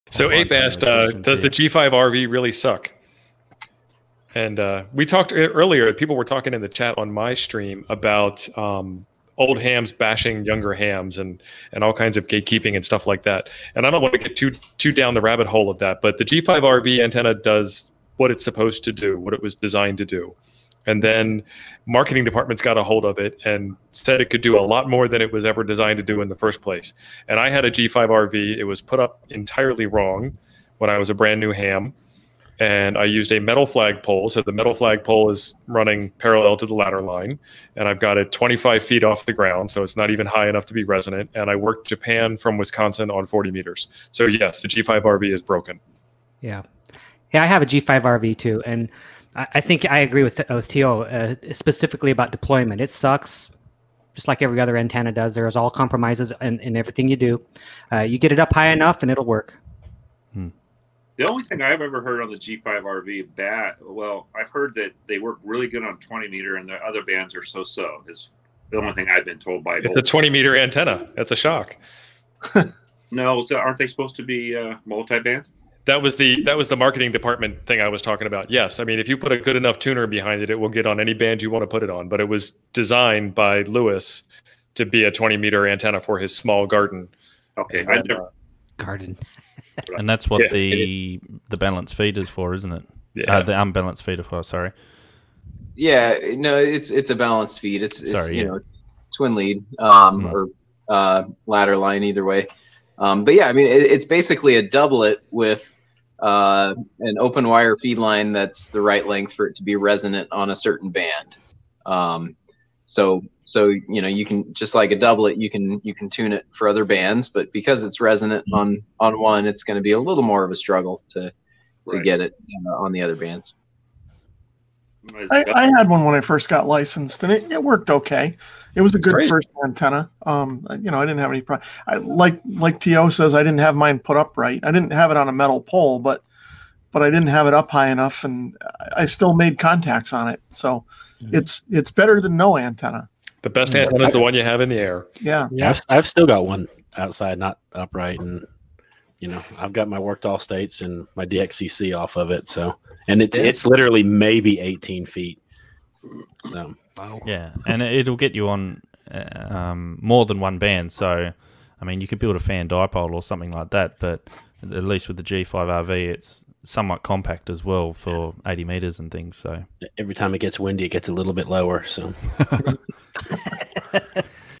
Does the G5RV antenna suck?
We've all heard of the famous G5RV antenna. But is it really all it's cracked up to be? How well do they work if at all? What do you need to avoid when deploying the antenna to ensure it works properly.

We answer "Does the G5RV suck?" on Ask an Elmer.